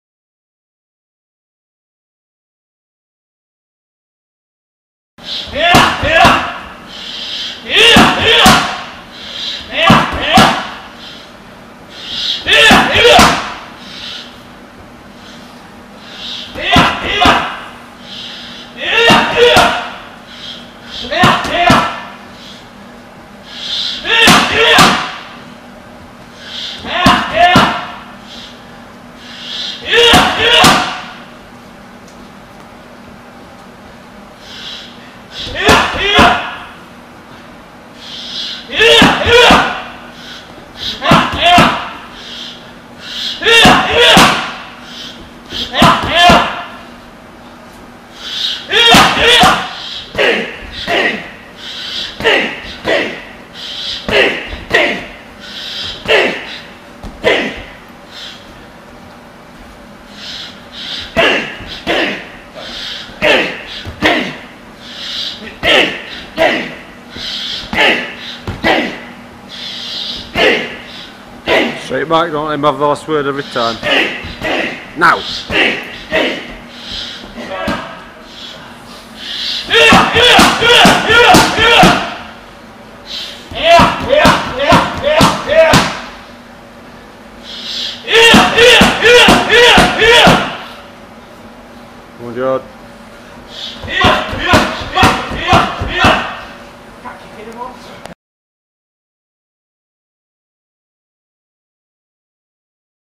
Liam Harrison & Jordan Watson Warm Up
Liam and Jordan warming up before sparring.